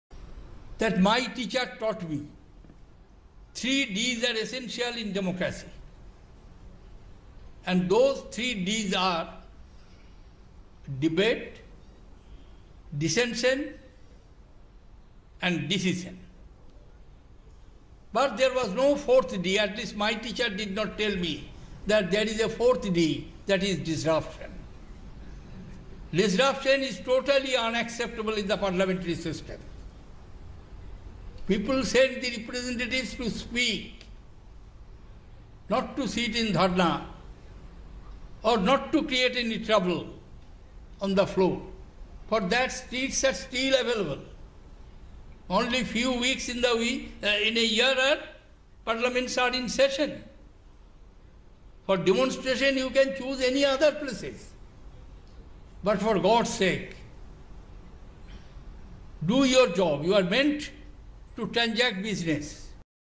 President Pranab Mukherjee at a meeting of Defence Estates Organisation
Disruption of Parliament is not acceptable at all: President Pranab Mukherjee says at a meeting of Defence Estates Organisation